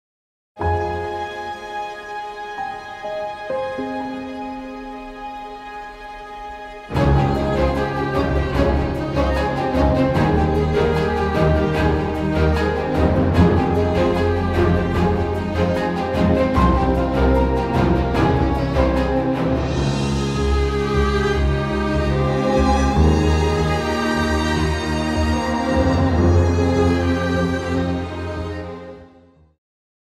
「中岡慎太郎館」PR動画
坂本龍馬の盟友と云われる幕末の志士「中岡慎太郎」の故郷高知県北川村に建つ、慎太郎の生涯と偉業を顕彰する資料館『中岡慎太郎館』のPR動画。